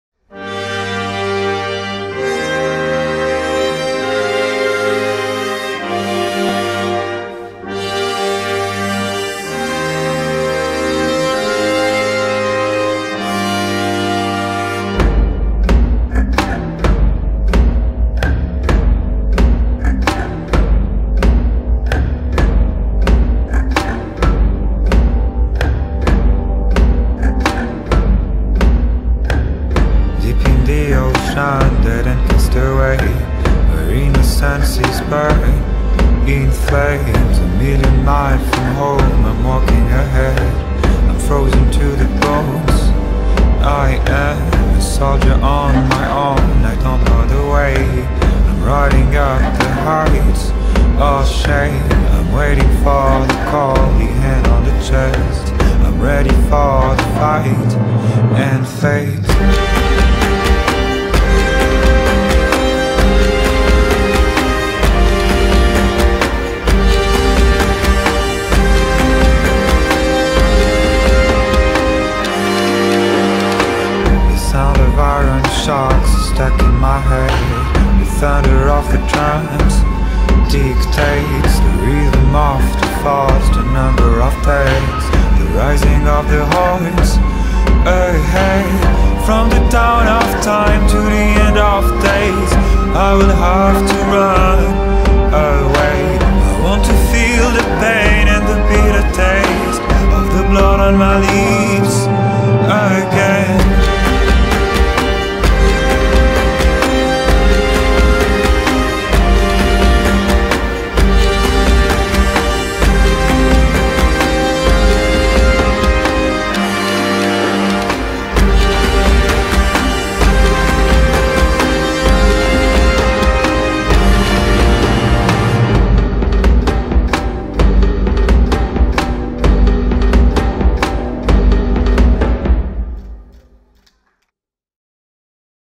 PG 11A kl.- ,,Ferrum’’, juh. Daiana Hanna Madjar, Elsbeth Heinaste
Paide Gümnaasiumi võimlemispidu 2014, filmis Saundland OÜ